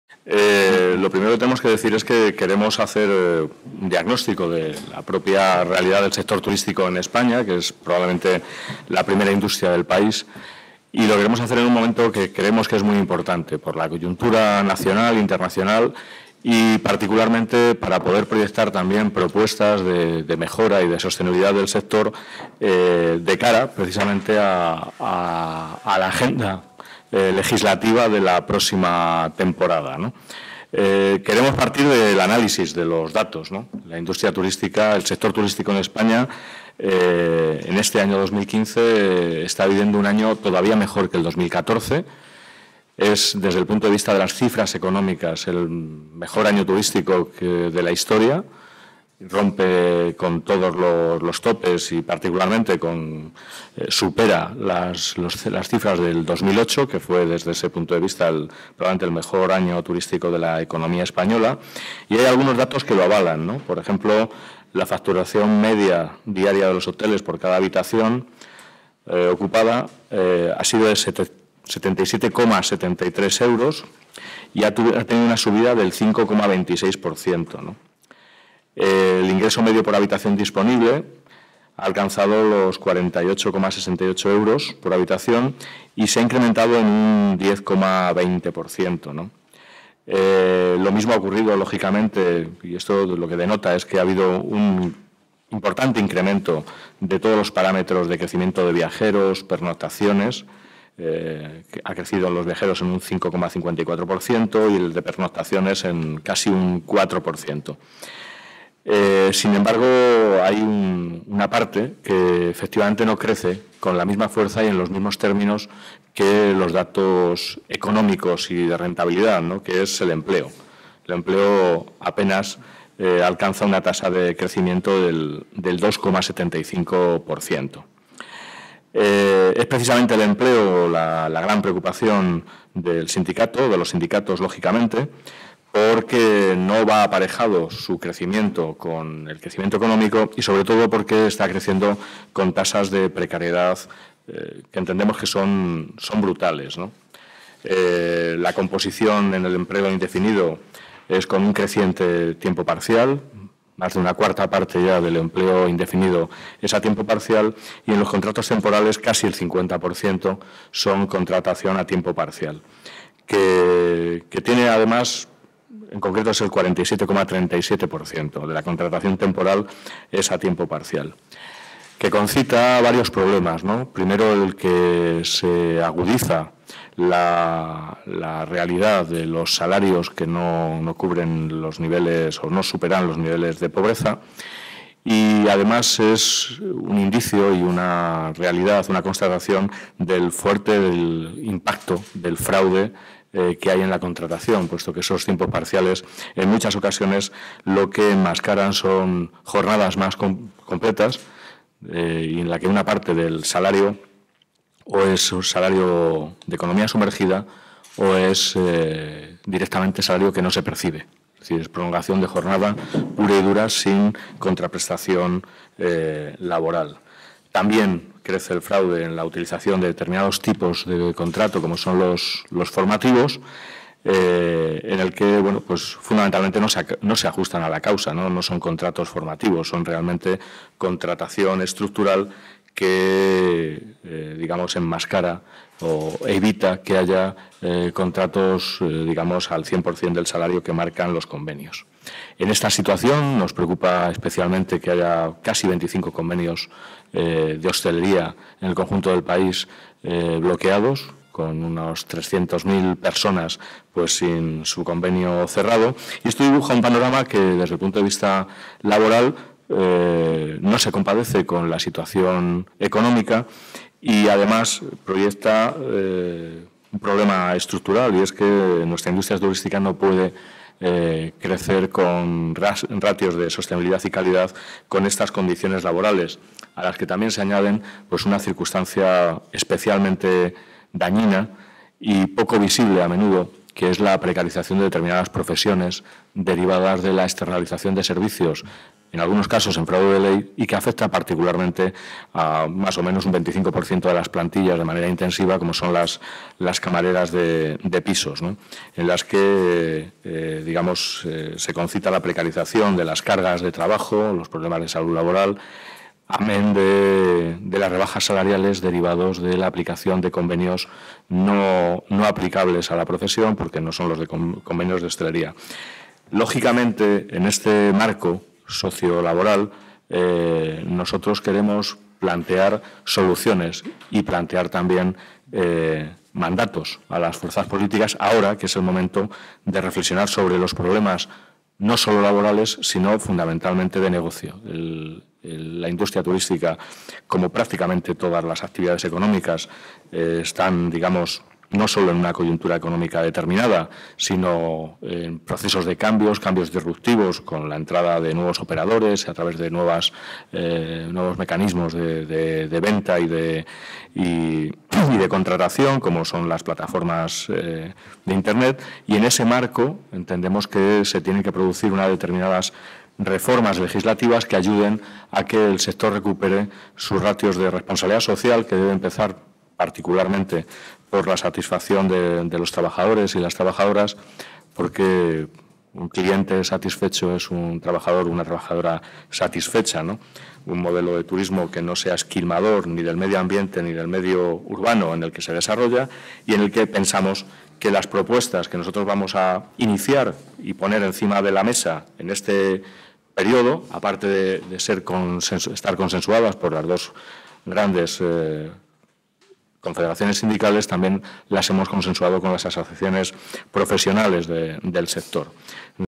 Propuestas para una Industria Turística de Calidad
Intervención de José María Martínez, Secretario General de Servicios CCOO, en la rueda de prensa para presentar las propuestas de CCOO y UGT para una industria turística de calidad.